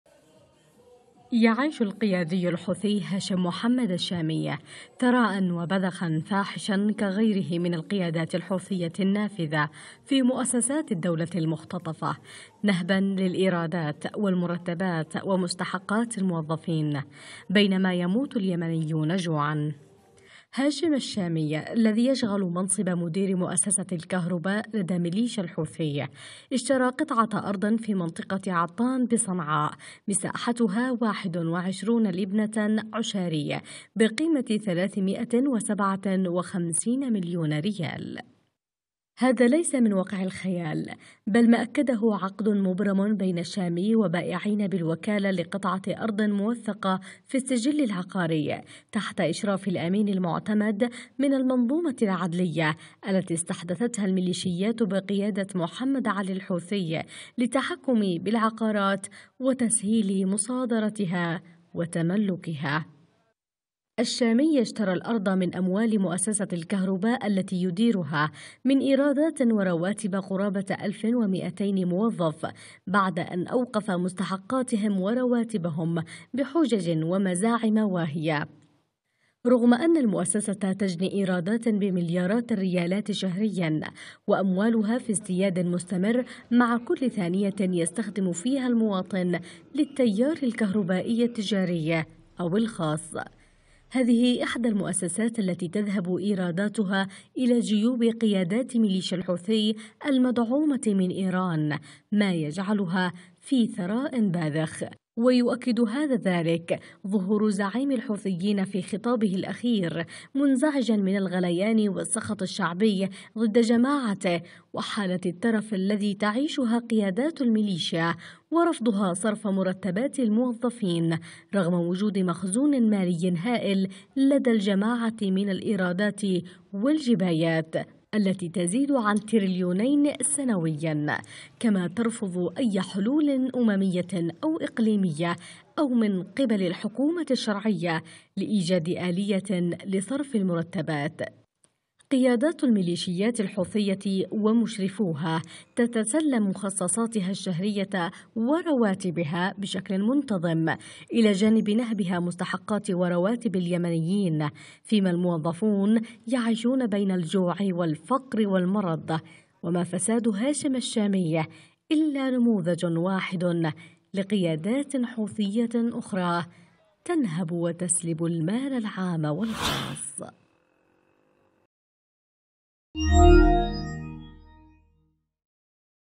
هاشم الشامي.. نموذج ثراء فاحش لقيادات الحوثي من أموال اليمنيين
قناة فضائية يمنية مستقلة تبث على مدار النايلسات بالترددات التالية :
HD     11257/H/27500
SD     11391/V/27500